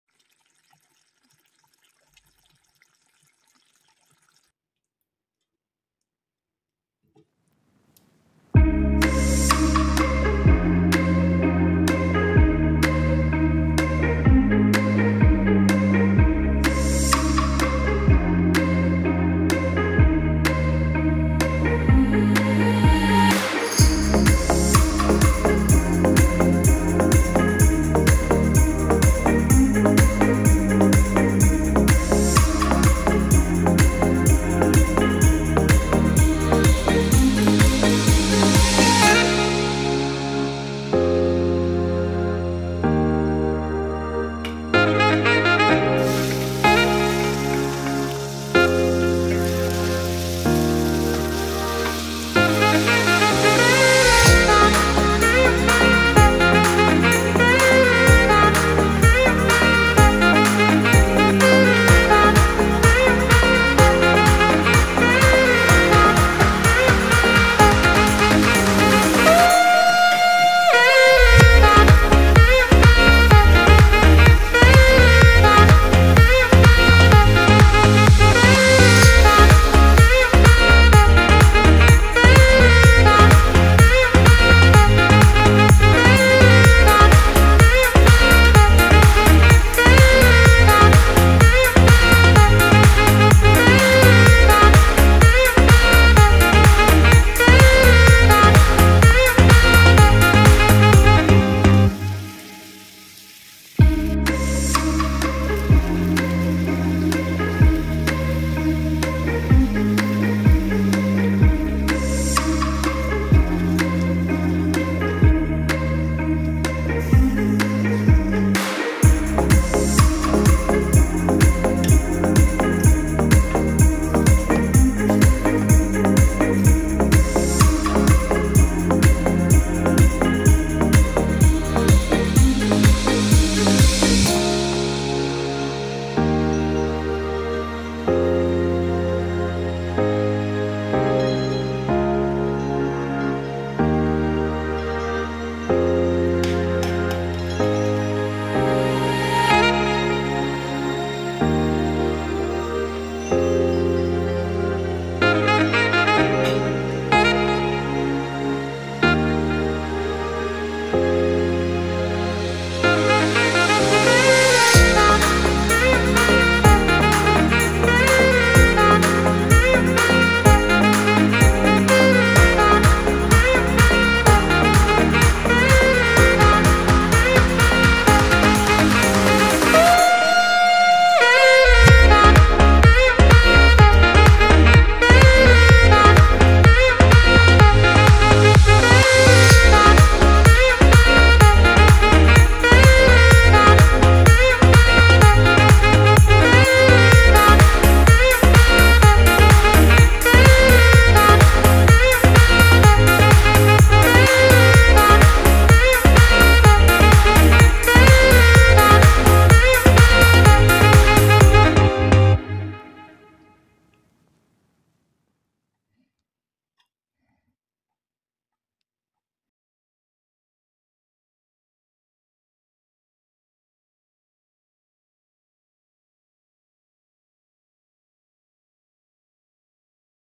Bicol Express ( The Most Expensive )
Nilagayan ko ng gold Ang bicol Express 😂 KC may golden onions..

Musician: LiQWYD